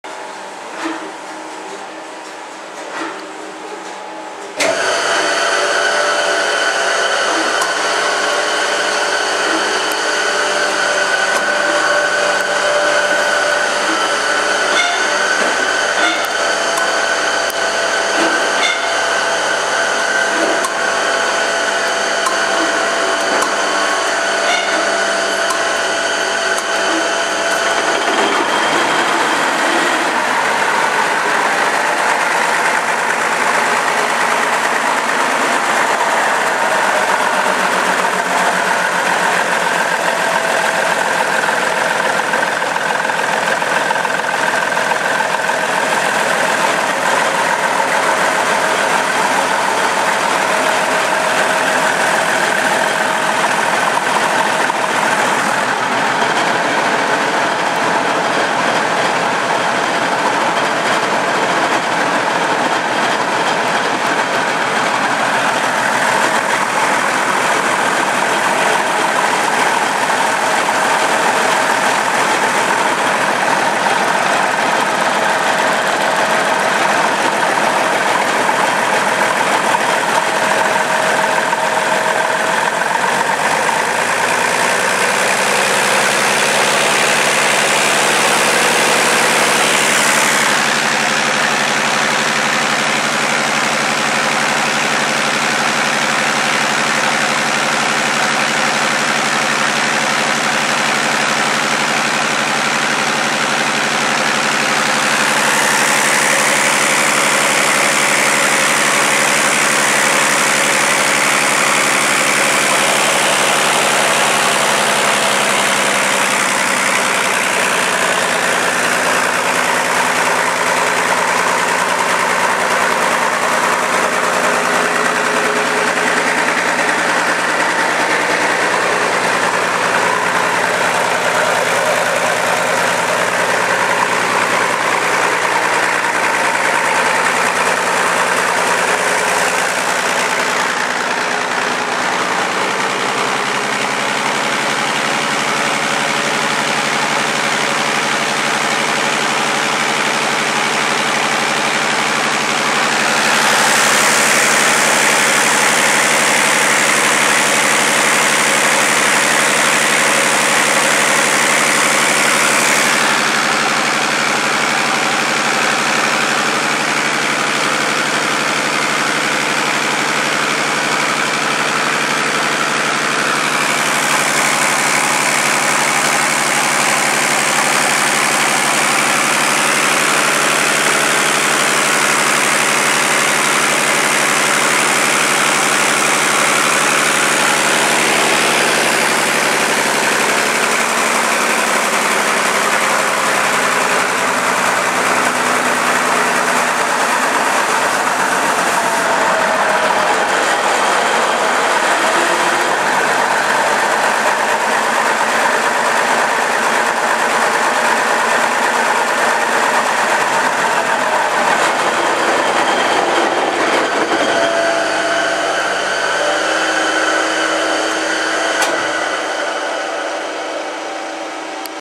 Предварительная обкатка двигателя DEUTZ BF6M1015 на обкаточном стенде без заводки
Показан процесс предварительной обкатки двигателя на обкаточном стенде без его заводки. На момент съёмки двигатель был прогрет до температуры 70-80 град. Окончательная обкатка будет произведена непосредственно на его штатном месте и под нагрузкой.